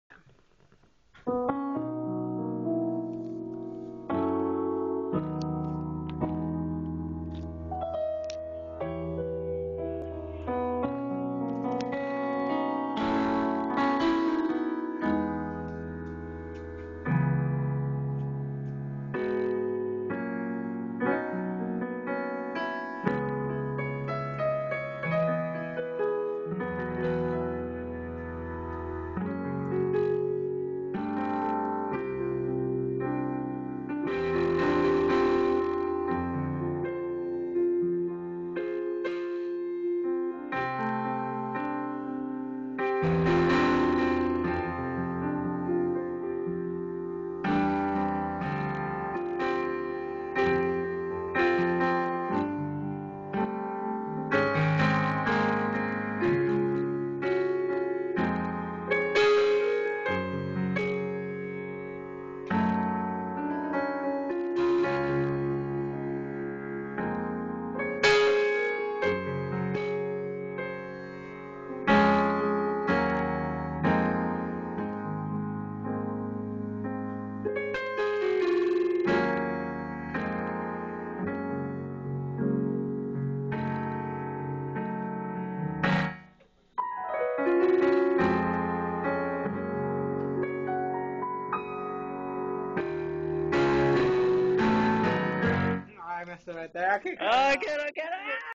mannee on keys
my boy mannee messing around on keys (COGOP Riverside)